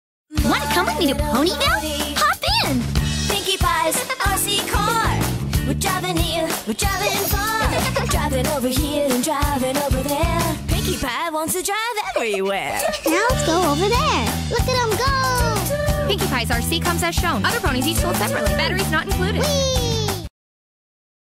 Pinkie Pie RC Car Toy (TV Commercial) | My Little Pony Toys for Kids | Friendship is Magic
BEEP! BEEP! Look out! PINKIE PIE zooms into town in her car! Even if she's in a hurry, she always has time to give her friends a ride. Hop in and go for a spin with PINKIE PIE! PINKIE PIE keeps her pony friends laughing and smiling all day! Cheerful and playful, she always looks on the bright side. Pinkie Pie's Remote Control Car comes with a Pinkie Pie figure, her car, and remote control. Purchase Pinkie Pie's pony friends' figures so she can ride around Ponyville with her friends!

Download the My Little Pony App!